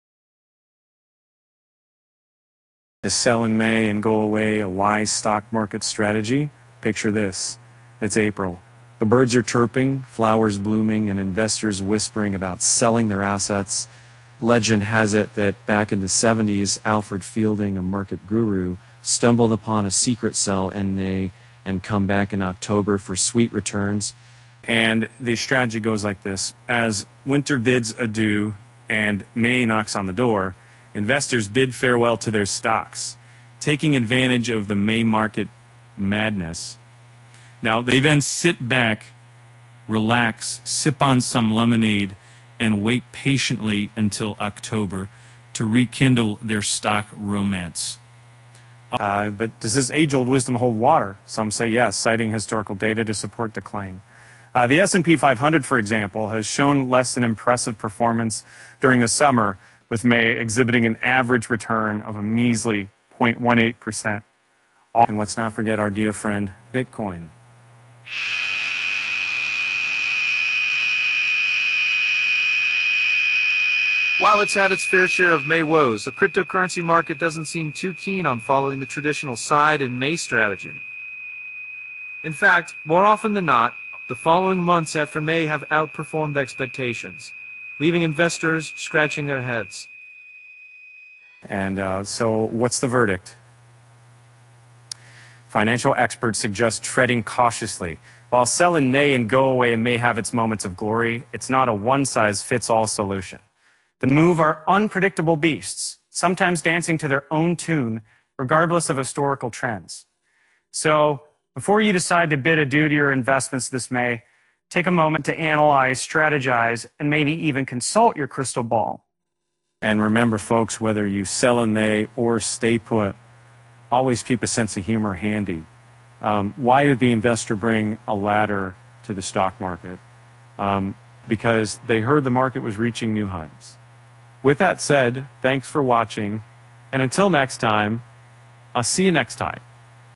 What is Sell in May and Go Away?
"Is 'Sell in May and Go Away' a Wise Stock Market Strategy?" Picture this: it's April, the birds are chirping, flowers blooming, and investors whispering about selling their assets. Legend has it that back in the '70s, Alfred Fielding, a market guru, stumbled upon a secret - sell in May and come back in October for sweet returns.